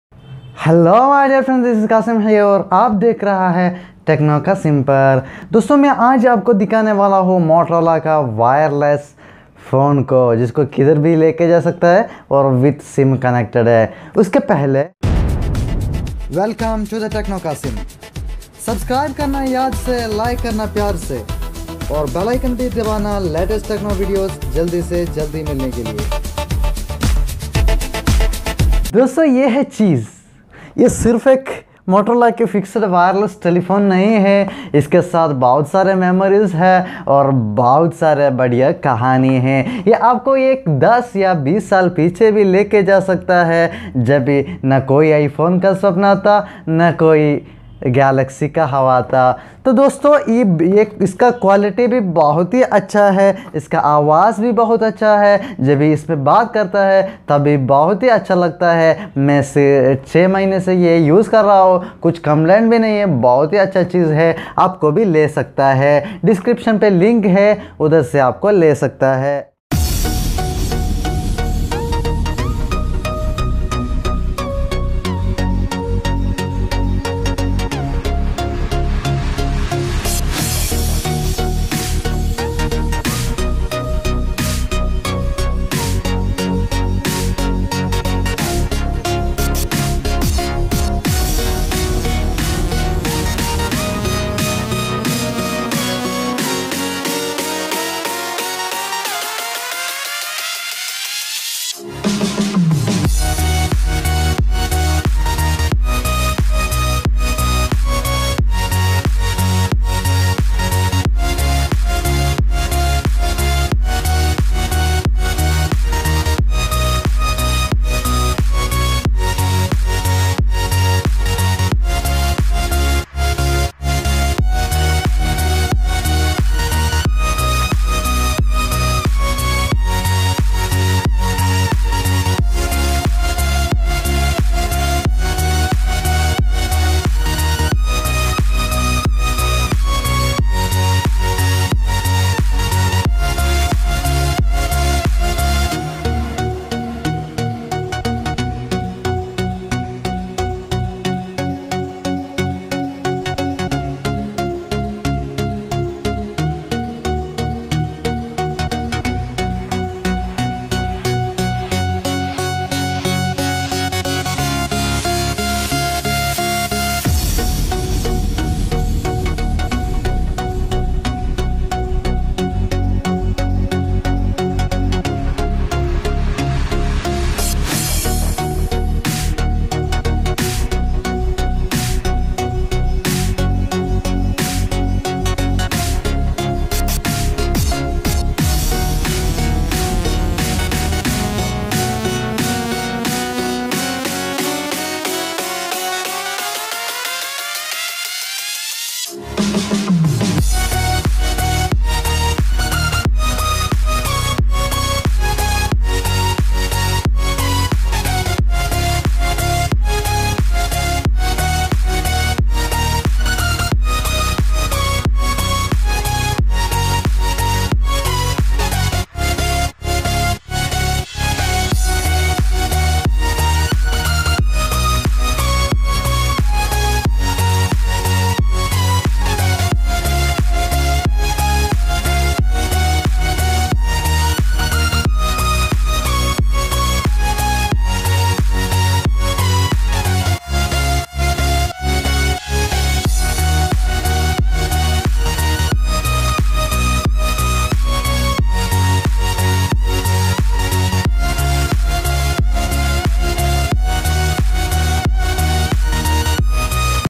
UNBOXING MOTOROLA LANDLINE FIXED WIRELESS FW200L TELE PHONE
UNBOXING MOTOROLA LANDLINE FIXED WIRELESS FW200L PHONE

ye Motorola wireless telephone ka unboxing video hei. aapko online se kareedsaktha hei. aapko bahuth pasand hojayega,

Camera stand clip: